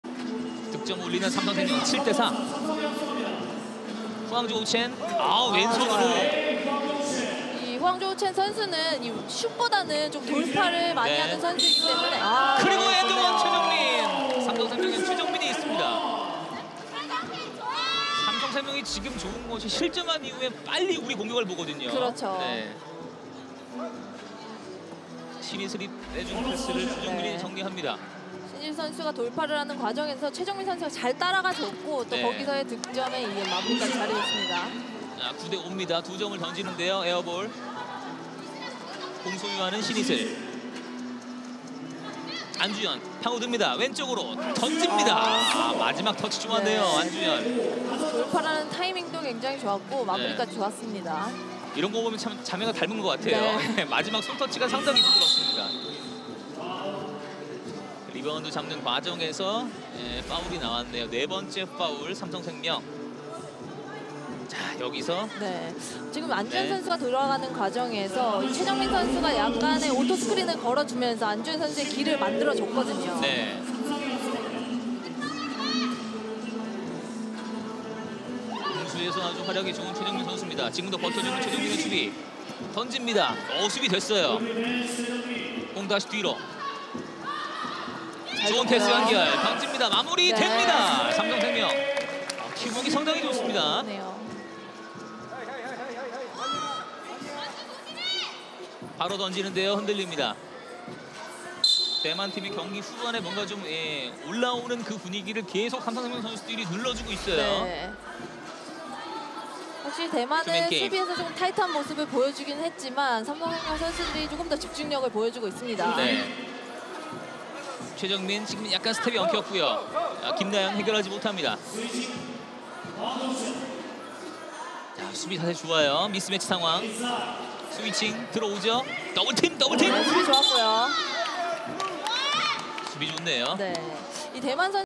[WKBL여자농구트리플잼] - [대만 vs 삼성생명] 경기 초반부터 리드하는 삼성생명.20190929
[대만 vs 삼성생명] 경기 초반부터 리드하는 삼성생명.